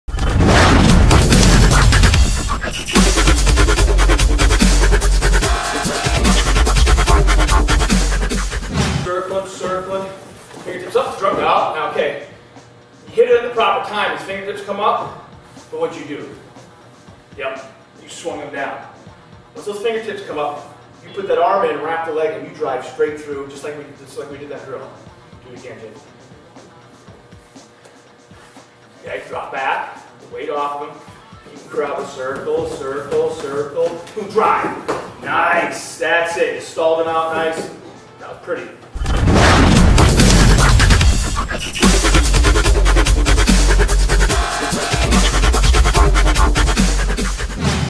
Dresser Dump
The Dresser Dump is a great move that requires exact timing, feel, and technique. Once all these things come together, you have a hold that can produce falls against the toughest opponents.

Pressure is the key to a Dresser Dump. Instead of forcing the maneuver, the offensive wrestler releases pressure to bait his opponent into a poor position. From here the attacker establishes an angle and explodes to big points.

The Dresser Dump is a deadly hold that presents itself in virtually every match! Don’t leave those points on the table.